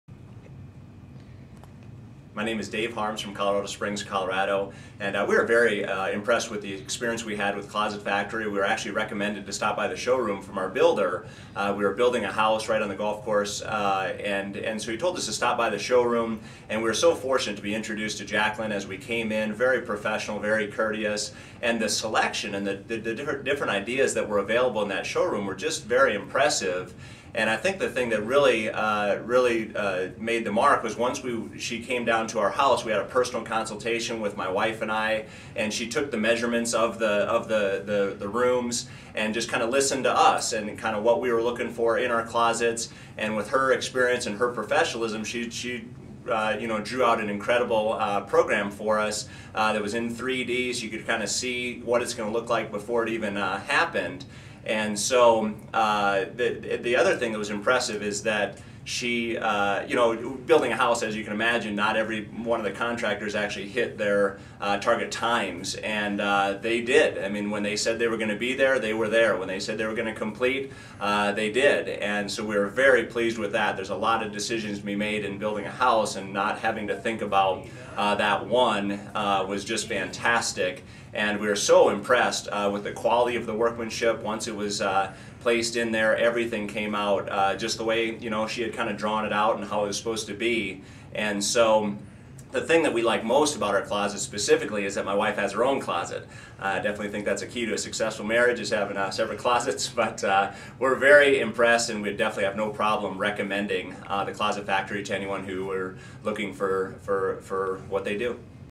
Colorado - Closet Factory Review - Harms
Dave Harms was recommended to Closet Factory through his builder and visited the showroom in Centennial, CO to get a better feel for what we do. There he met designer Jacqueline Lewis who took them through the entire design and installation process. Now he and his wife have separate, organized closet- his key to a successful marriage.